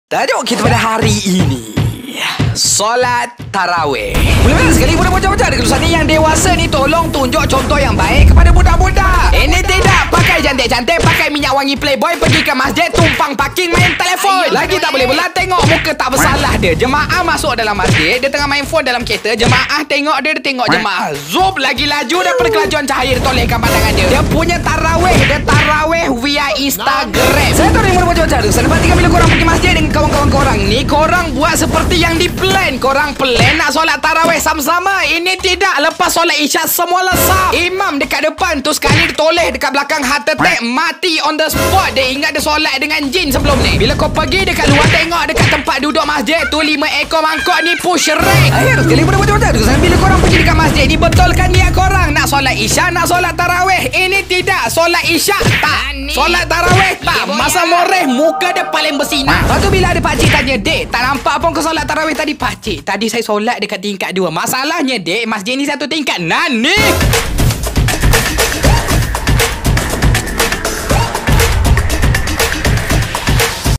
Kata nak tarawih, tapi pergi masjid PUSH RANK
Solat Tarawih
Sehari je dia solat tarawih HAHAHA